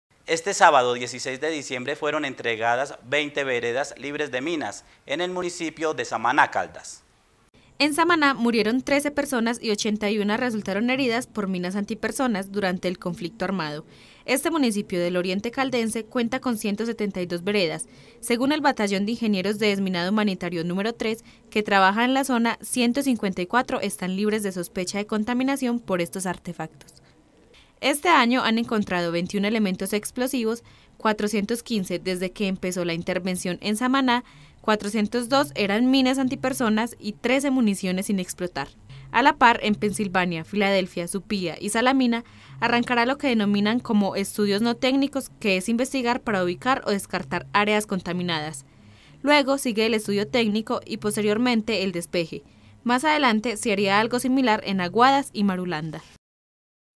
ENTREGAN 20 VEREDAS LIBRES DE MINAS EN SAMANÁ
Este sábado 16 de diciembre fueron entregadas 20 veredas libres de minas en el municipio de Samaná Caldas.